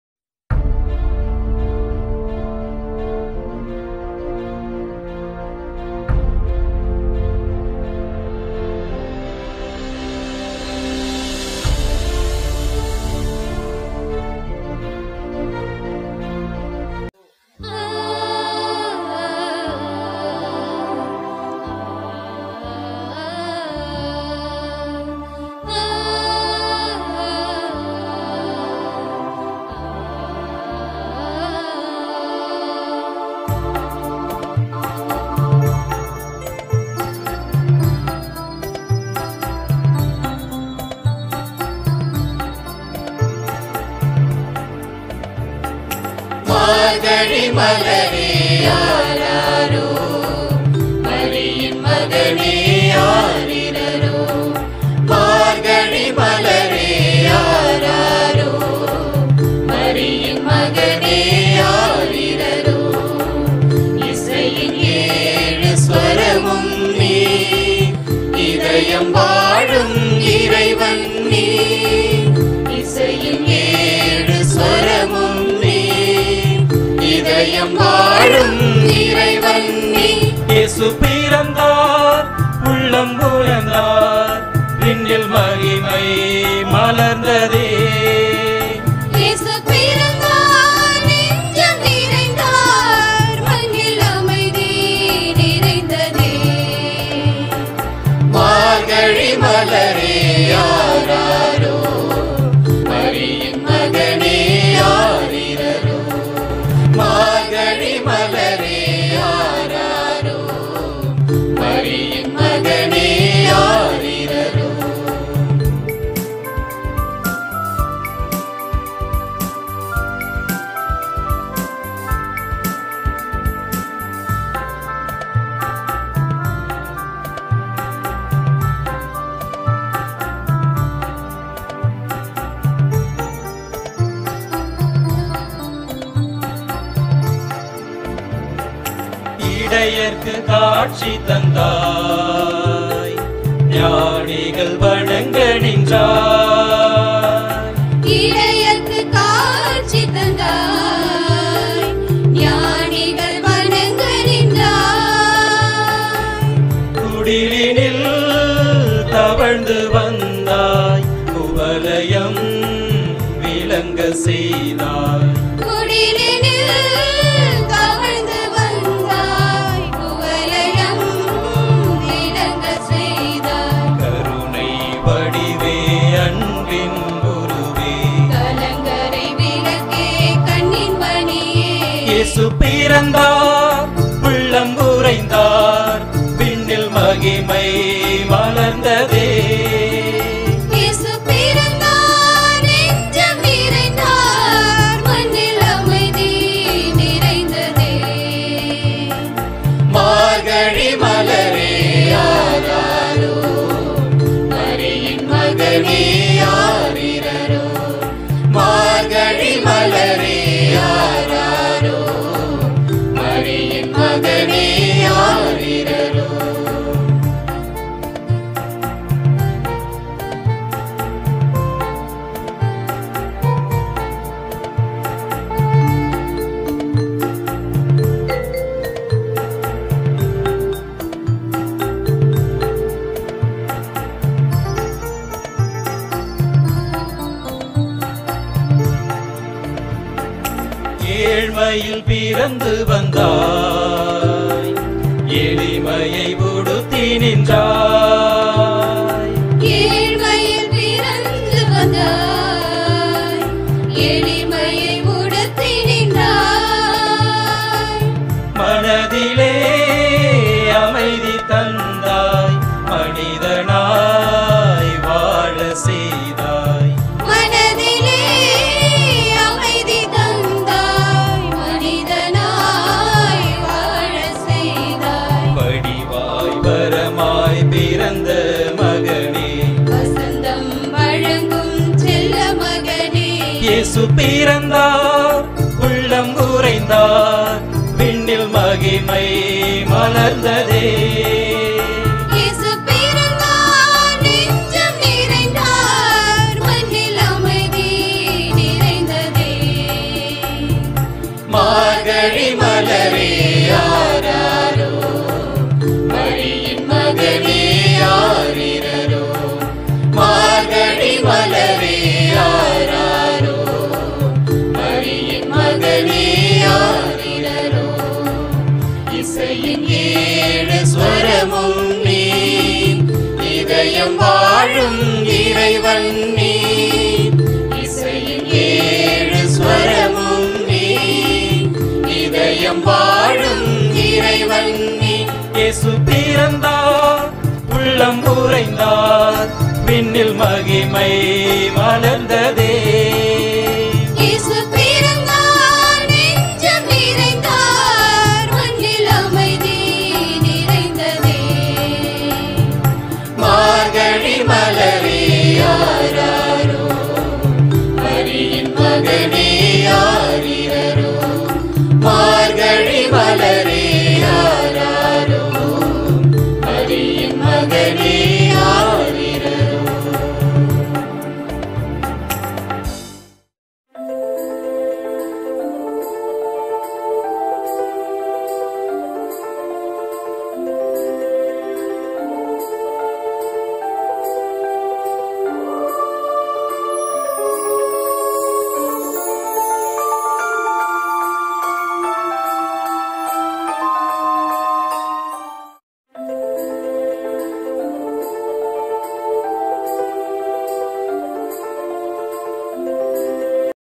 Tamil Christmas Carols 2021 - மார்கழி .....
Production: Archdiocese of Pondicherry – Cuddalore.
Keyboard: S.Johnpaul
Singers: Rev.Sr. Honorine Mary, R.Ratchagar, V.Vimalraj, D.James Raj, M.Pravin Paul, J.Philomindoss Antony John, M.Thamizharasan, D.Motcharakkini, G.Therese Bella, A. Victoria, M. Devamalar Violet, R.Catherine Mary, Mrs.Prasila, A.Kavitha Berthil Bernard, S.Preethi, S.Magalin
 @ARCHDIOCESE PONDICHERRY